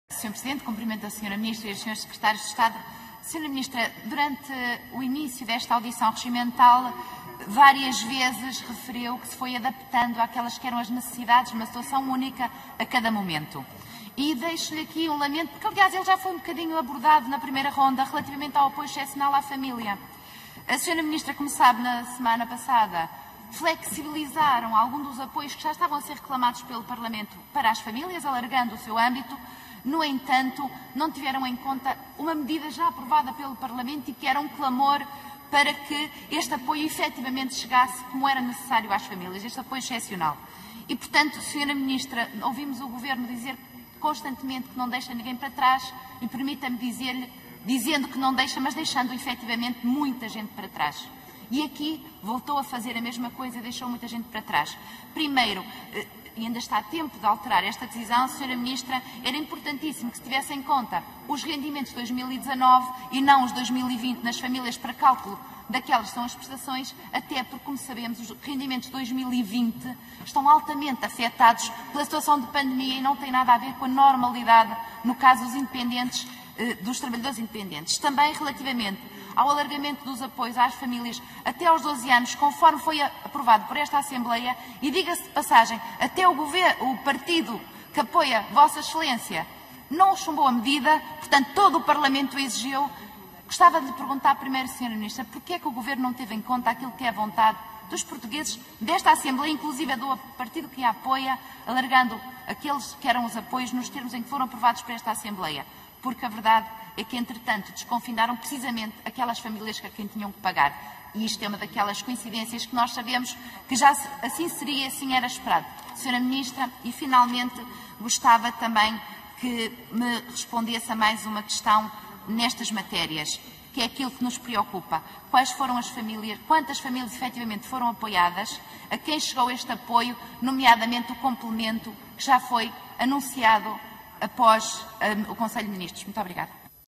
Emília Cerqueira questiona a Ministra do Trabalho, Solidariedade e Segurança Social
Intervenção na Comissão de Trabalho e Segurança Social.
17 março 2021